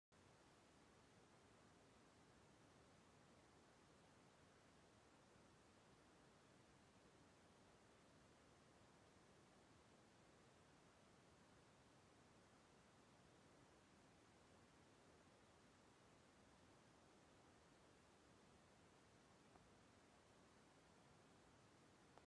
A lapful of kittehs!
This is what happens when I try to relax on the couch. My lap becomes cat central.